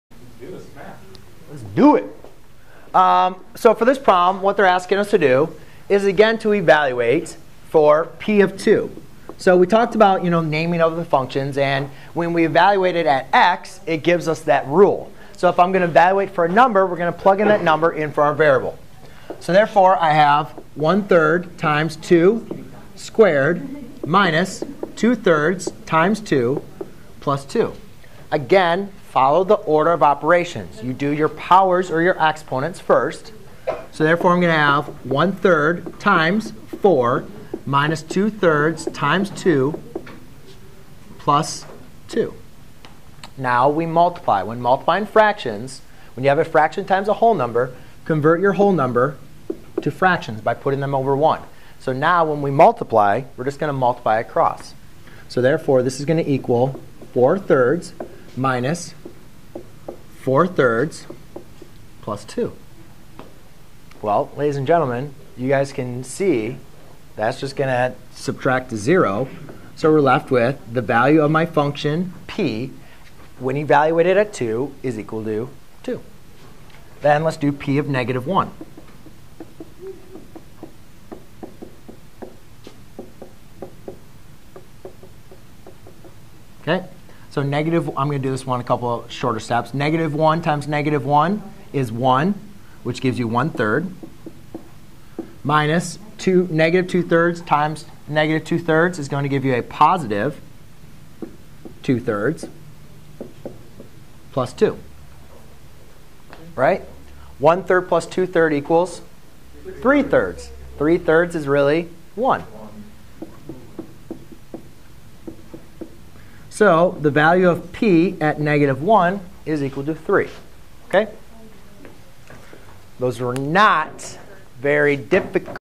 Learn how to evaluate a polynomial with fractions
👉 Learn how to apply the rules of exponents to simplify an expression.  We will focus on applying the product rule, quotient rule as well as power rule.  We will then explore multiple properties such as power to product, power to quotient and negative exponents.